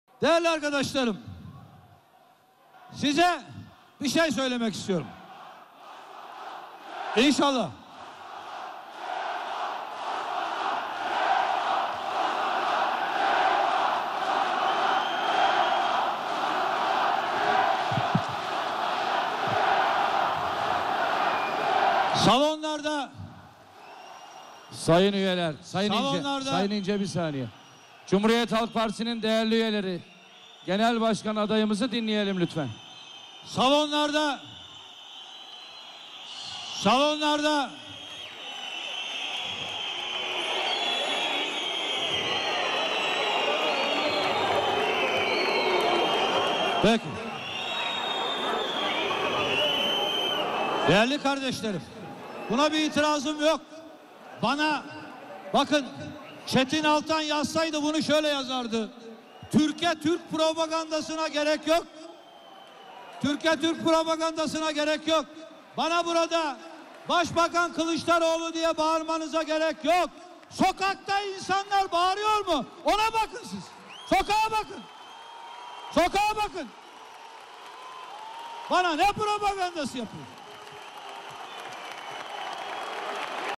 İNCE'DEN 'İNCE' MESAJ
CHP GENEL KURULU'NDA GENEL BAŞKAN ADAYI MUHARREM İNCE 'BAŞBAKAN KEMAL' SLOGANLARINA BÖYLE CEVAP VERDİ.